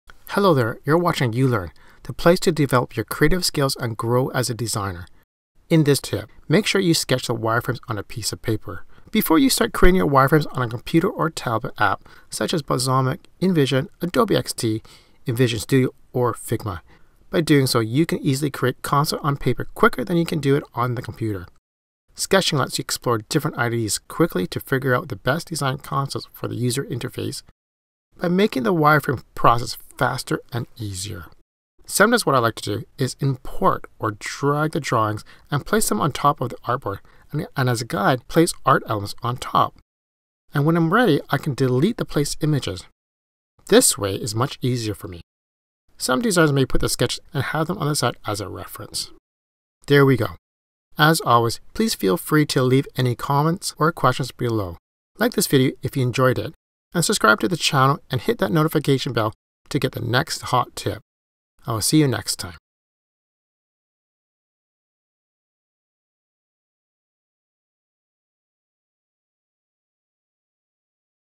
Sketch Wireframes on Paper First
Want to speed up your workflow in wireframes? Before you start creating your wireframes on a computer or tablet app; sketch the wireframes on a piece of paper. By doing so, you can easily create concepts on paper quicker than you can do it on the computer.

Sketching lets you explore different ideas quickly to figure out the best design concepts for the user interface by making the wireframe process faster and easier. Afterwards, you can either scan the wireframes or take a photo of them to place it in the app.

⏰  TIMESTAMPS:
00:00 Intro to Hot Tip
00:22 Sketching Makes Things Easier
00:31 Place Sketch or Drawings in the App as a Reference
00:49 Thanks for watching ending

🎓 If you love the channel but want to learn even more, read our blog for in-depth tutorials

Let's get started and have some fun!

ulearn.tech